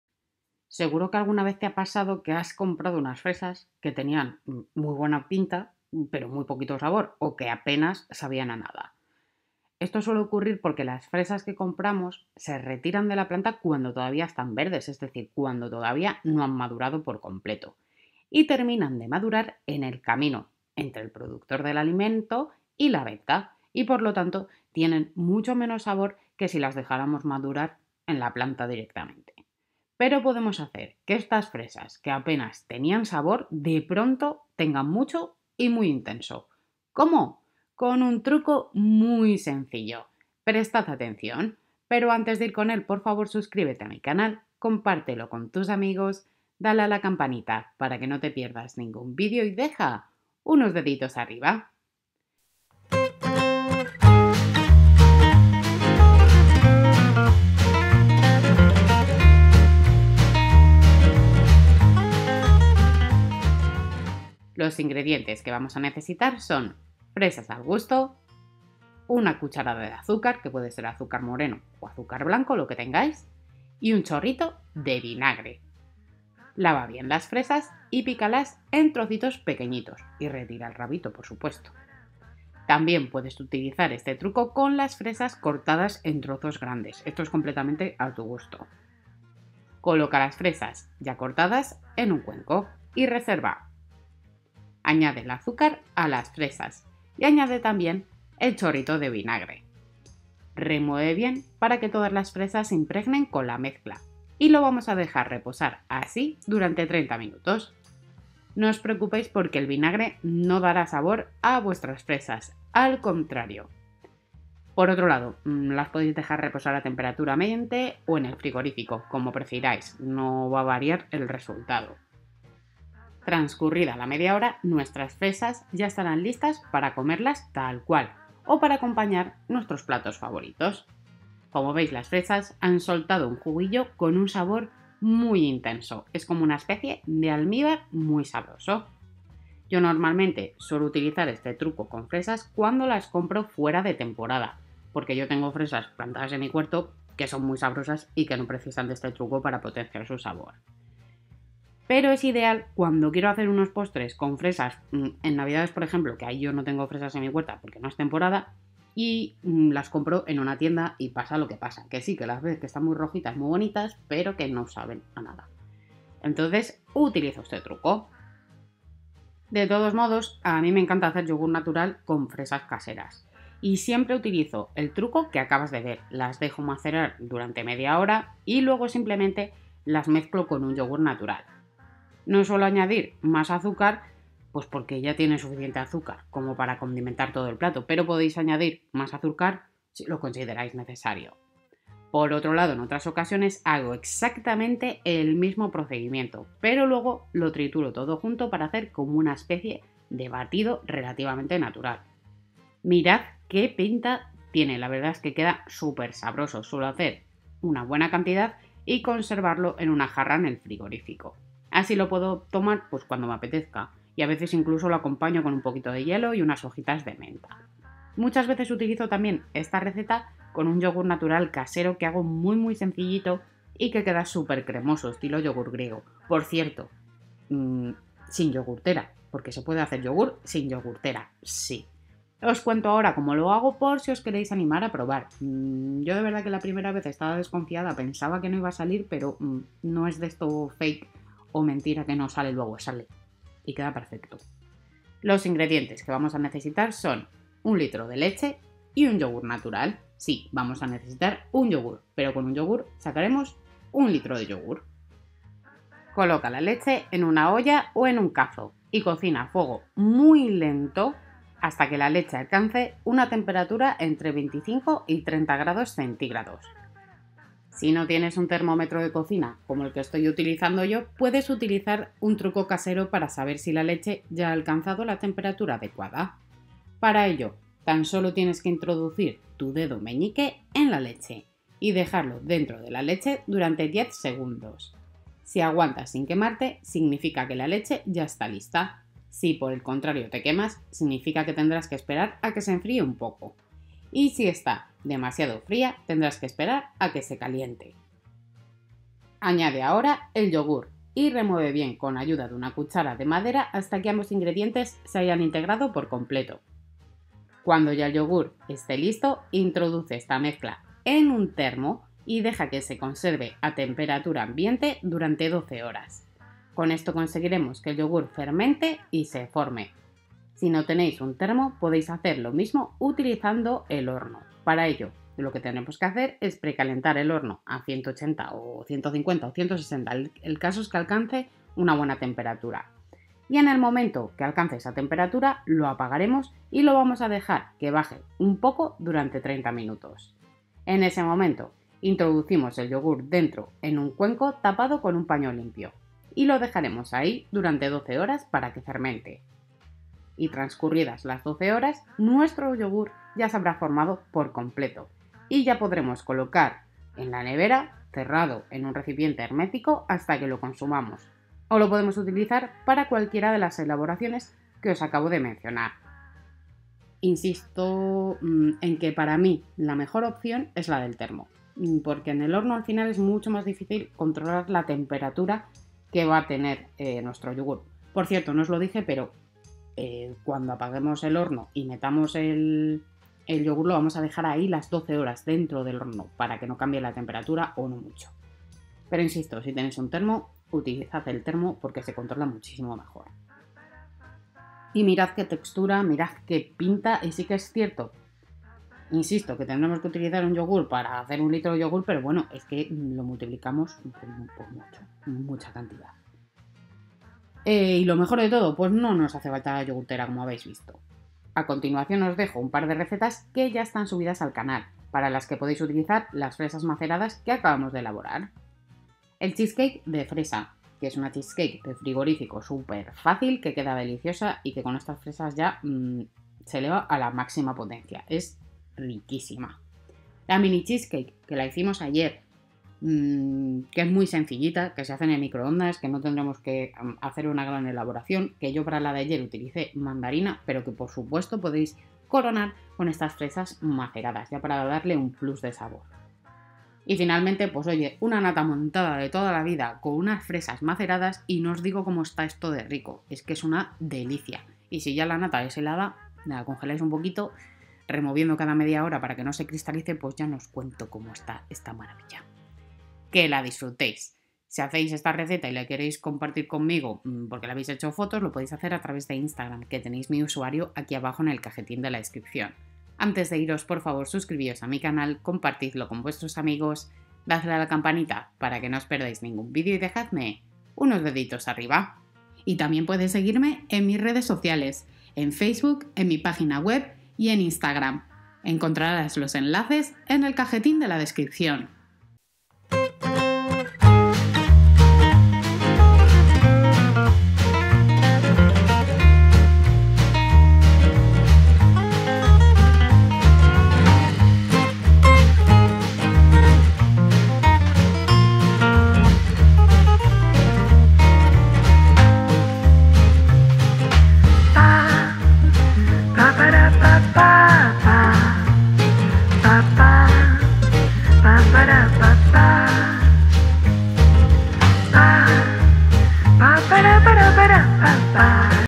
Truco Fácil para Intensificar el Sabor de las Fresas en Minutos 🍓 ¡Funciona Perfecto!
¿Alguna vez te ha pasado que compras fresas y descubres que no tienen el sabor dulce e intenso que esperabas? Este es un problema muy común y suele ocurrir porque las fresas se recogen antes de estar completamente maduras. Aunque maduren en el camino desde el productor hasta tu hogar, su sabor nunca llega a ser tan rico como si hubieran madurado en la planta. Pero no te preocupes, ¡tengo la solución perfecta para ti!

En este video, te voy a enseñar un truco casero súper sencillo que hará que esas fresas insípidas recuperen su sabor de una manera espectacular. No importa si ya te has dado cuenta de que no tienen el sabor que deseas; este truco hará que las fresas se vuelvan más dulces y sabrosas, como si las hubieras recogido en su punto perfecto de maduración. Lo mejor de todo es que es un método rápido, fácil y con ingredientes que seguramente ya tienes en casa.

Además, este truco es ideal no solo para fresas de temporada, sino también para aquellas fresas que compras fuera de temporada y que, a menudo, tienen un sabor más débil. Con este consejo, podrás disfrutar de fresas deliciosas en cualquier época del año, ya sea para acompañar un desayuno saludable, como postre o simplemente para comerlas frescas.

No te pierdas este video lleno de tips prácticos para mejorar el sabor de tus fresas. Te aseguro que una vez pruebes este truco, no querrás volver a comer fresas sin antes aplicarlo. ¡Tus papilas gustativas te lo agradecerán!

Si te gustan este tipo de consejos prácticos para mejorar la calidad de tus alimentos, asegúrate de suscribirte a mi canal y activar la campanita de notificaciones para no perderte ningún video. ¡Es totalmente gratis!👇